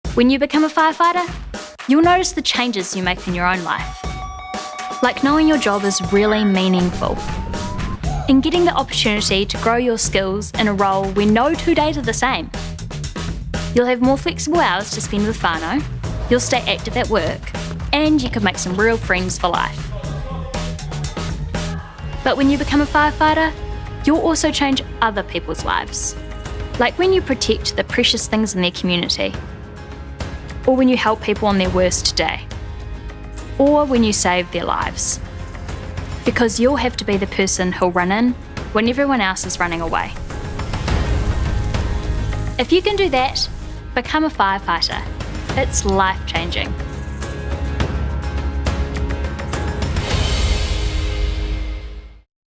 Become a Career Firefighter. It's Life Changing.
A career as a firefighter is about protecting the community. You’ll notice the changes to your own life. But you’ll also change other people’s lives.

Become a firefighter. It’s life changing.